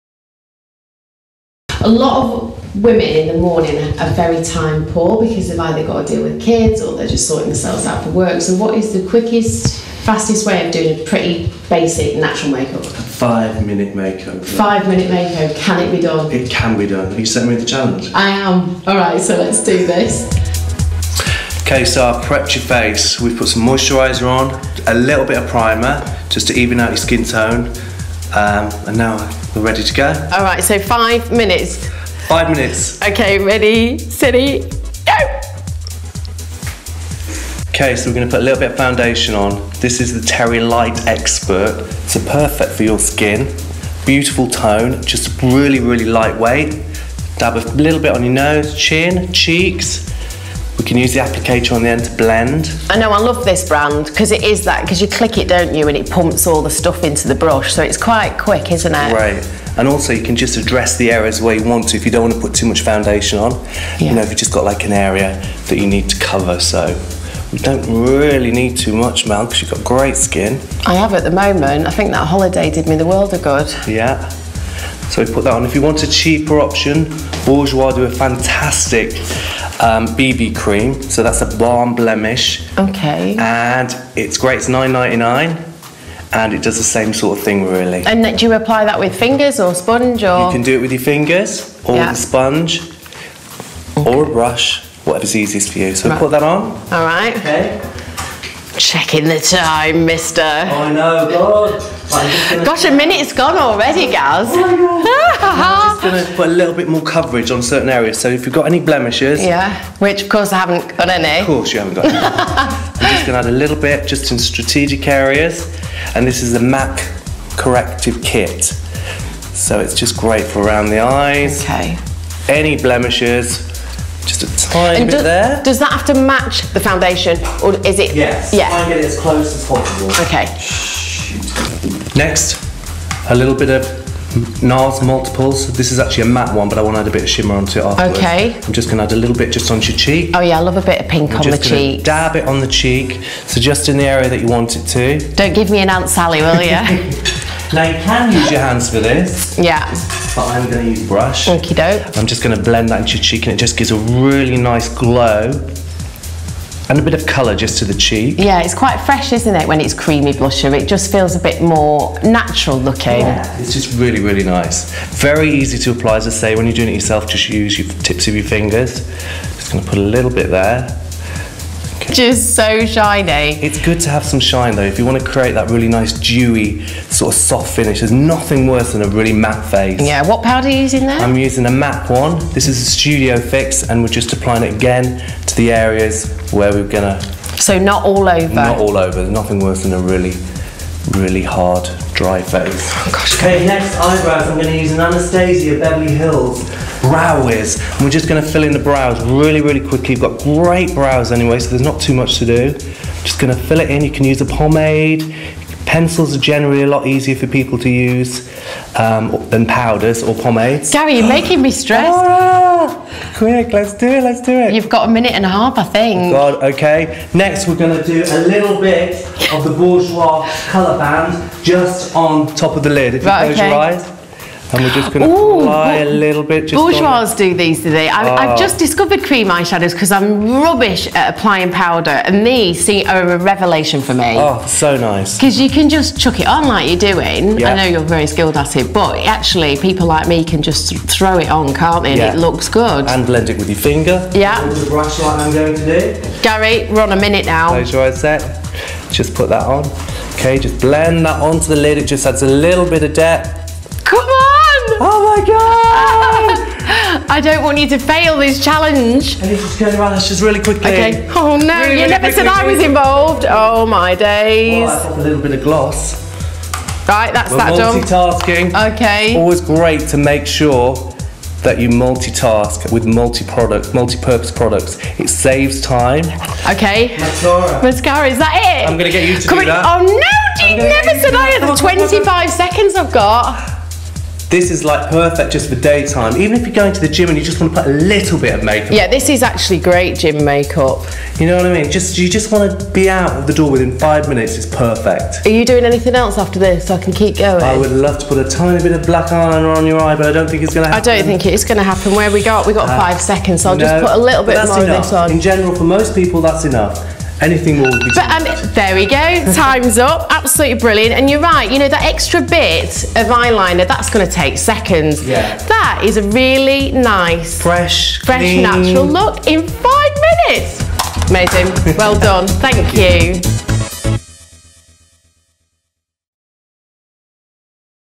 Melanie Sykes - The 5 Minute Makeover with Gary Cockerill
Melanie runs through a quick five minute makeover with makeup artist Gary Cockerill.